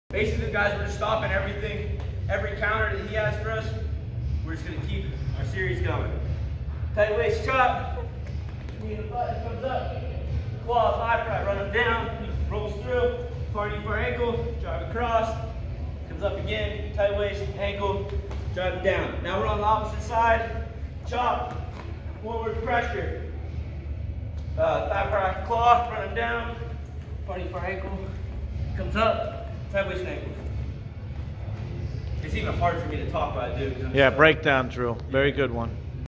Breakdown Drill part 1
Wrestling technique taught by D1 wrestler and Pennsylvania State Champ